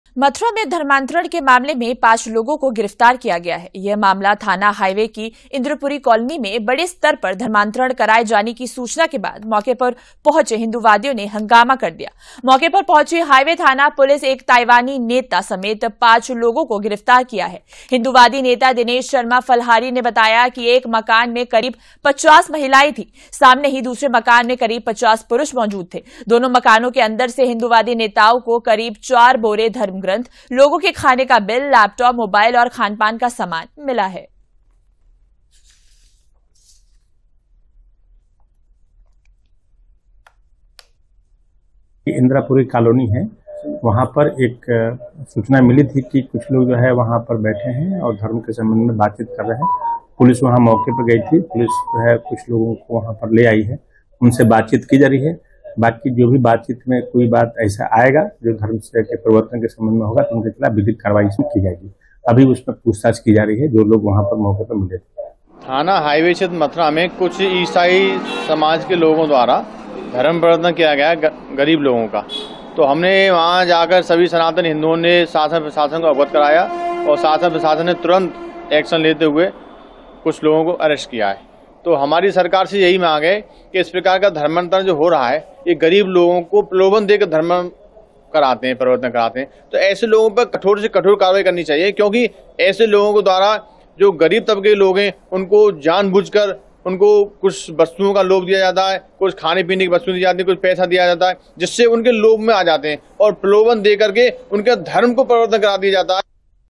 Mathura धर्मांतरण मामले में एक ताइवानी नगारिक समेत 5 लोग गिरफ्तार
मथुरा में धर्मांतरण के मामले में पांच लोगो को गिरफ्तार किया है। यह मामला थाना हाइवे की इंद्रपुरी कॉलोनी में बड़े स्तर पर धर्मांतरण कराए जाने की सूचना के बाद मौके पर पहुंचे हिंदूवादियों ने हंगामा कर दिया। मौके पर पहुंची हाईवे थाना पुलिस एक ताइवानी नेता/नागरिक समेत पांच लोगो को गिरफ्तार किया है। हिंदूवादी नेता दिनेश शर्मा फलाहारी ने बताया कि एक मकान में करीब 50 महिलाएं थी। सामने ही दूसरे मकान में करीब 50 पुरुष मौजूद थे। दोनों मकानों के अंदर से हिंदूवादी नेताओं को करीब चार बोरे धर्मग्रंथ, लोगों के खाने का बिल, लैपटॉप, मोबाइल और खानपान का सामान मिला।